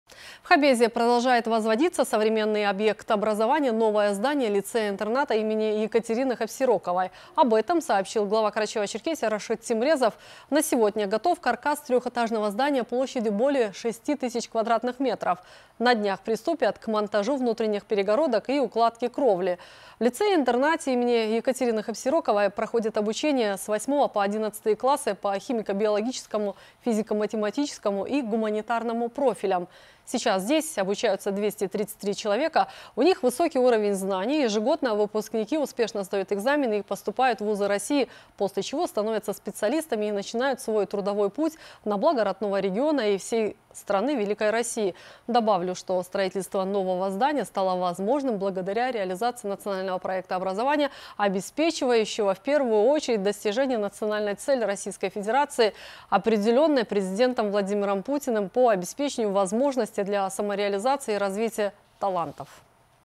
Рашид Темрезов рассказал о ходе строительства в Хабезе нового здания образовательного учреждения
Сегодня лицей-интернат имени Екатерины Хапсироковой в Хабезе - это место, где обучаются около 250 школьников 8-11 классов по трём профилям. Благодаря Национальному проекту "Образование" было начато возведение нового здания учреждения. О значимости лицея в образовательном процессе и о ходе строительных работ рассказал глава Карачаево-Черкесии Рашид Темрезов:
"Хочу отметить высокий уровень знаний, которые получают дети: ежегодно выпускники успешно сдают экзамены и поступают в вузы России. На сегодня готов каркас трёхэтажного здания площадью более 6 тыс. кв.м. На днях приступят к монтажу внутренних перегородок и укладки кровли".